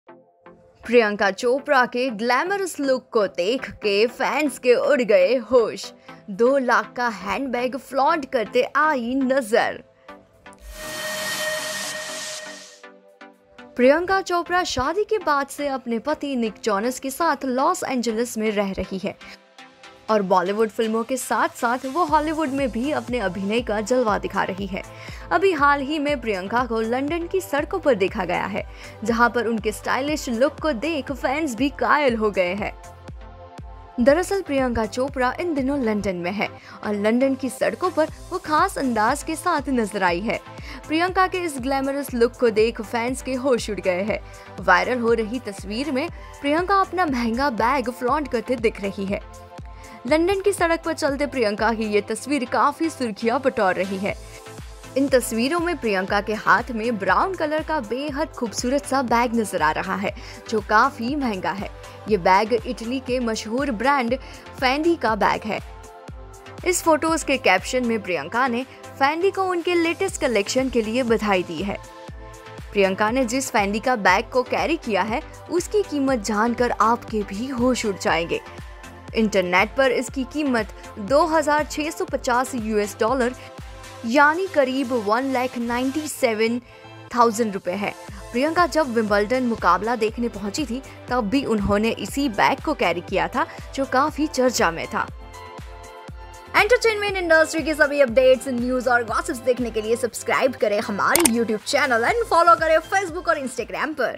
प्रियंका चोपड़ा के लुक को देख दंग रह गए फैन्स, 2 लाख के हैंडबैग को फ्लॉन्ट करती नगर आई ऐक्ट्रेस
Latest Bollywood News of Entertainment Industry only at Indianfilmhistory.com

Address:
1/3,Shere Punjab Colony,
Andheri East.
Mumbai 400 093
India.